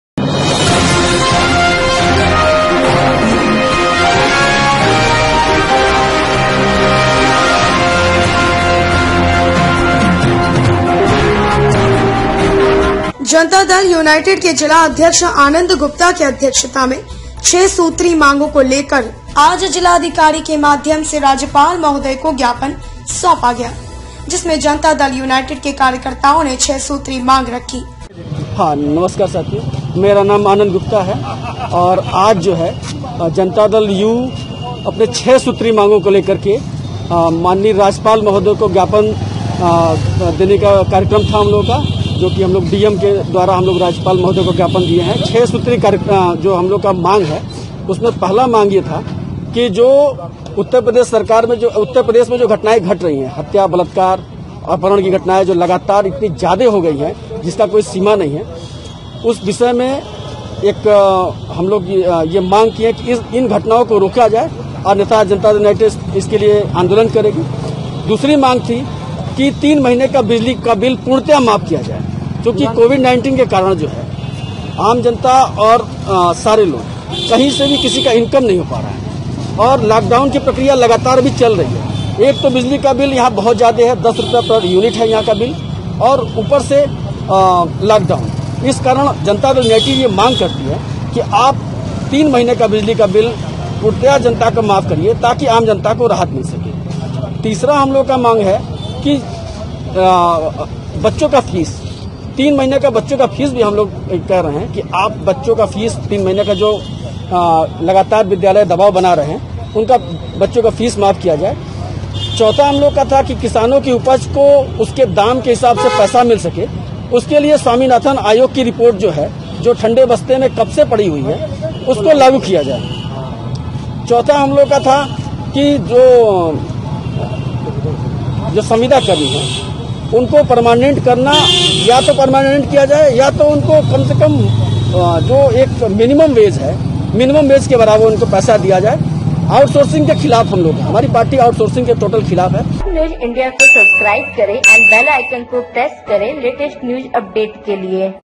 वाराणसी:- 6 सूत्री मांगों को लेकर के JDU जिलाधिकारी के माध्यम से राज्यपाल महोदय को ज्ञापन सौंपा गया
वाराणसी।

 जनता दल यूनाईटेड  के जिला अध्यक्ष आनंद गुप्ता के अध्यक्षता में 6 सूत्री मांगों को लेकर के आज जिलाधिकारी के माध्यम से राज्यपाल महोदय को ज्ञापन सौंपा गया जिसमें जनता दल यूनाईटेड के कार्यकर्ताओं ने 6 सूत्रीय  मांग रख्खी।

 पहला मांग
 प्रदेश में सरकार कानून व्यवस्था को दुरुस्त करने के लिए ठोस कार्रवाई करें

दूसरा मांग

प्रदेश सरकार सभी नागरिकों को 3 माह की बिजली का बिल माफ करें प्रदेश सड़क

 तीसरा मांग

 प्रदेश सरकार सभी छात्र छात्राओं से वर्तमान सत्र की निजी स्कूलों द्वारा मनमानी फीस वसूली पर तत्काल रोक लगाएं तथा प्रदेश सरकार कोरोना काल में तत्काल शराब बंदी लागू करें

 पांचवा मांग

 संविदा एवं आउटसोर्सिंग पर कार्यरत कर्मचारियों को स्थाई करें

 छठवां मांग

 किसानों की उपज का मूल्य दिलाने के लिए स्वामीनाथन रिपोर्ट सरकार लागू करें इन्हीं आज नीलकंठ तिवारी मुन्ना सिंह मनोज मिश्रा अंकित गुप्ता गुड्डू खान फिरोज अहमद आदि कार्यकर्ता जिलाधिकारी के माध्यम से ज्ञापन सौंपा।

🔹Anchor:- Ragini Rai
🔹Video Editor and Graphics:- Mohit kumar
🔹Social Media & IT Management:- Diksha jaiswal

shaurya news india